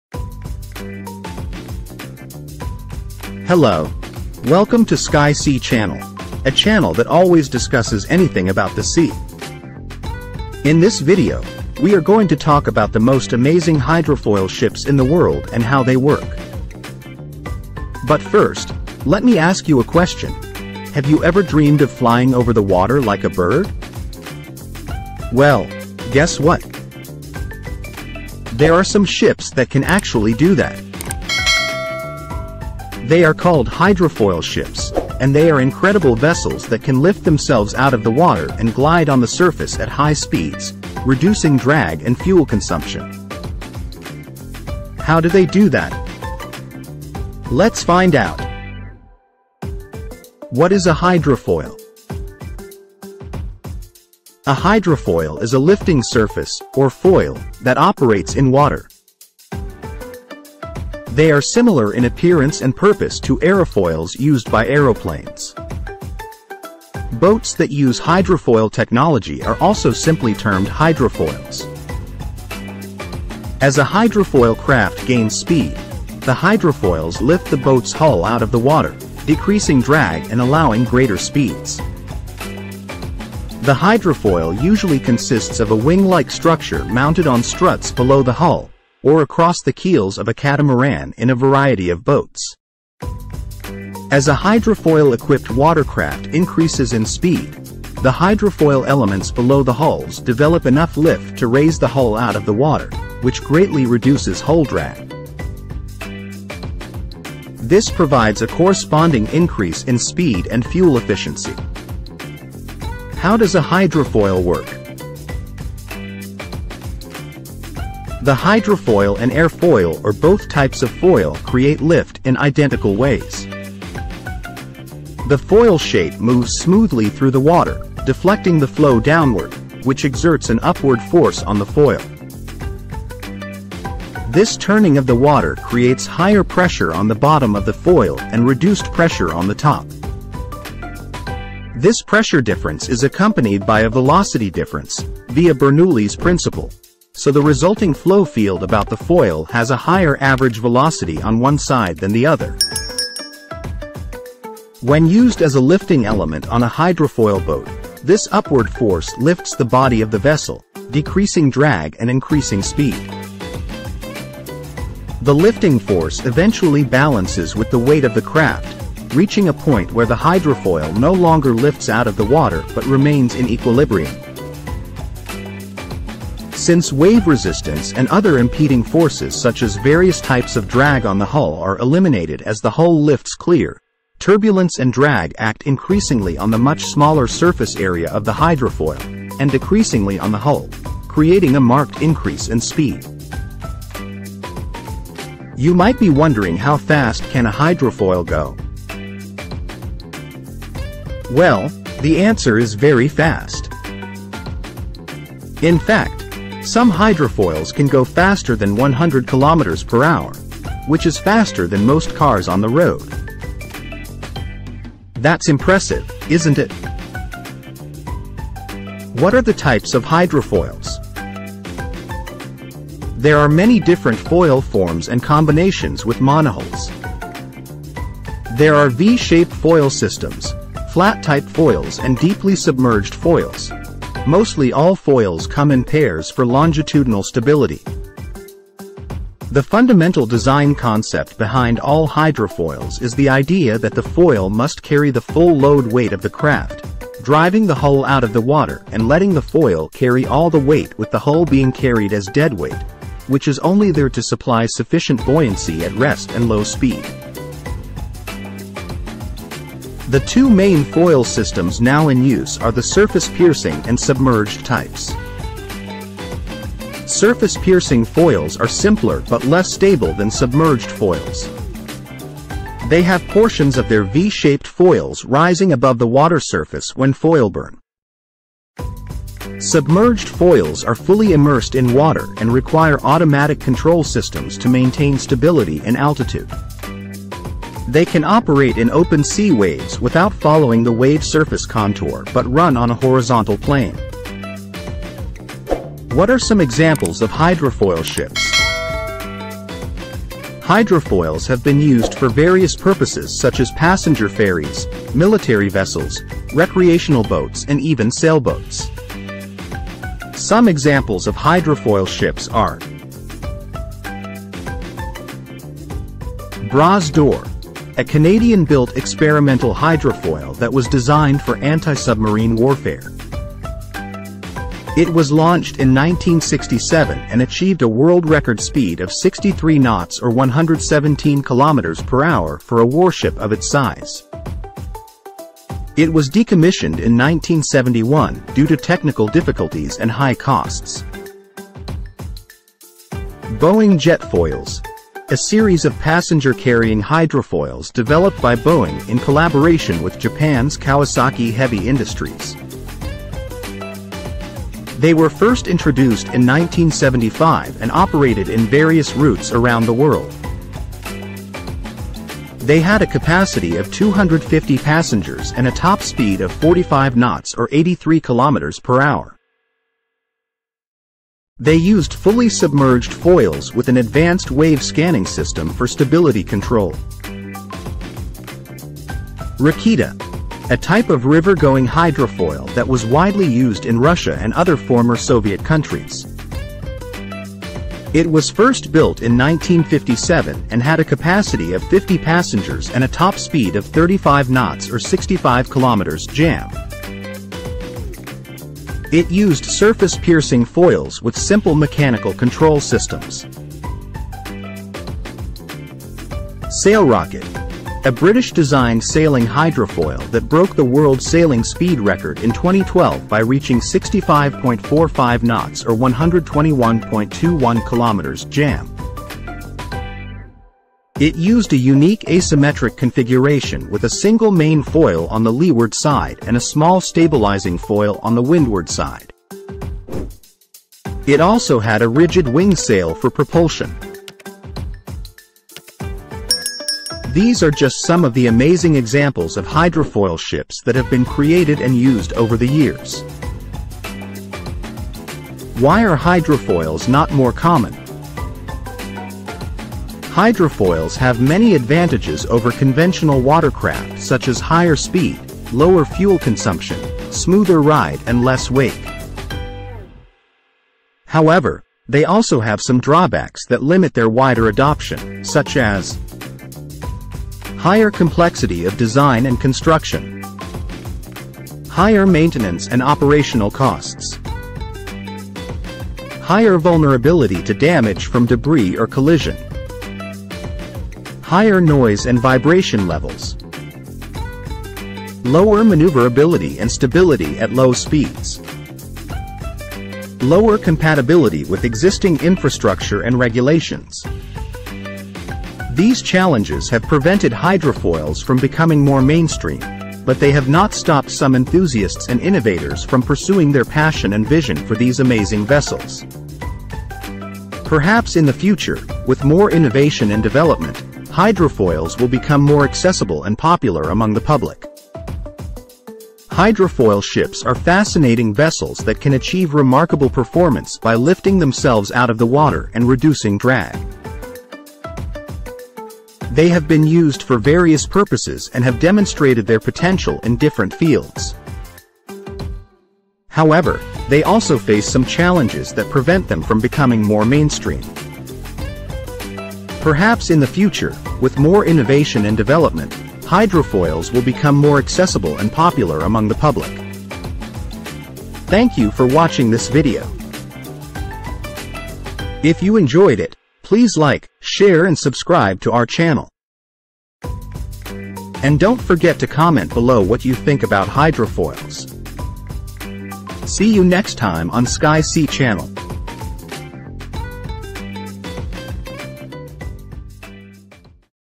How to Fly Over Water with Hydrofoil Ships
Have you ever dreamed of flying over the water like a bird? Well, guess what? There are some ships that can actually do that! They are called hydrofoil ships, and they are incredible vessels that can lift themselves out of the water and glide on the surface at high speeds, reducing drag and fuel consumption. How do they do that? In this video, we will explain what hydrofoil ships are, how they work, what are the types and examples of hydrofoil ships, why they are not more common, and what are the future prospects of these amazing vessels. If you want to learn more about hydrofoil ships and other amazing things about the sea, make sure to watch this video until the end!

00:00 -- Intro & Question
00:50 -- What is a hydrofoil?
01:53 -- How does a hydrofoil work?
03:15 -- How fast can a hydrofoil go?
03:38 -- What are the types of hydrofoils?
04:56 -- What are some examples of hydrofoil ships?
05:15 -- Bras d’Or
05:43 -- Boeing Jetfoils
06:18 -- Raketa
06:45 -- Sailrocket
07:24 -- Why are hydrofoils not more common?
08:35 -- Conclusion
09:07 -- Outro